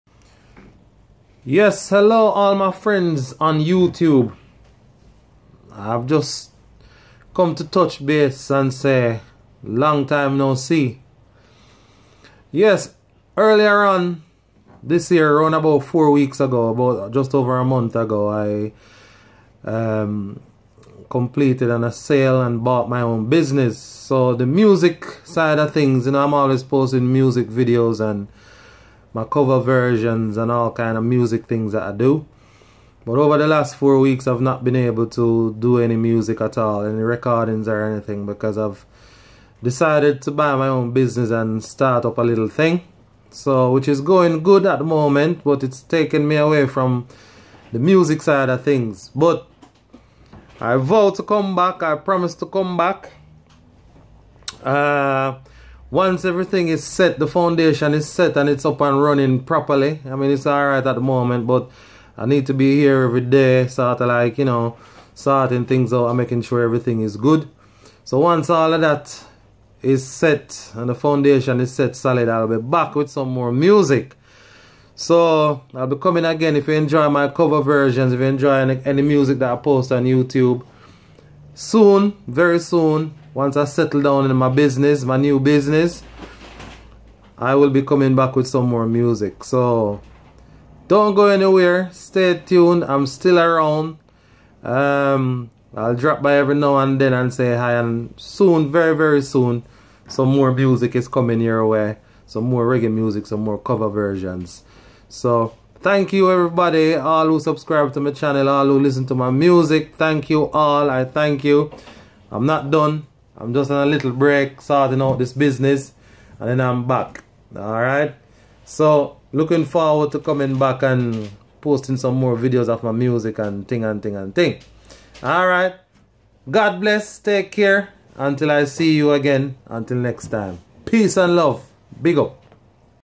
Bought my own business..but coming back soon.
I have had to take a break from YouTube recently due to buying my own business and working hard to keep it floating..I will be back soon with some more Reggae music once the foundation of my business is set solid peace and Love.